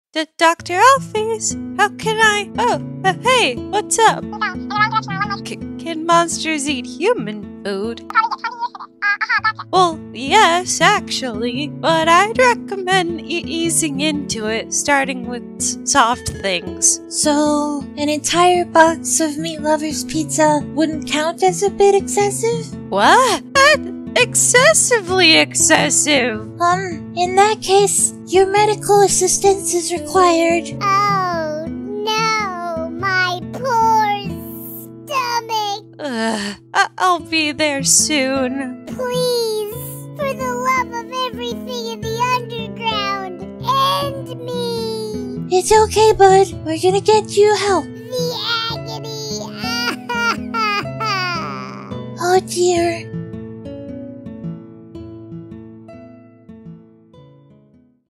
Medical Emergency! Asriel Frisk and Alphys! Undertale Comic Dub!
Frisk has a very important question for Dr alphys. It seems that Asriel has eaten a whole box of pizza...and theres a medical emergency to deal with!